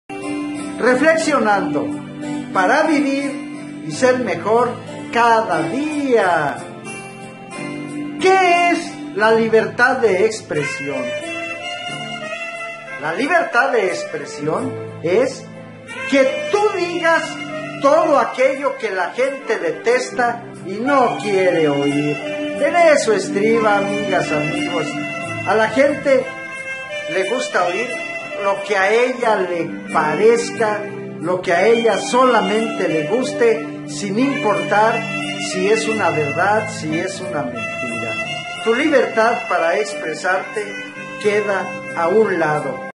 Libertad de expresión es que digas lo que a la gente no le gusta oír
Reflexiones con Francisco Camarillo López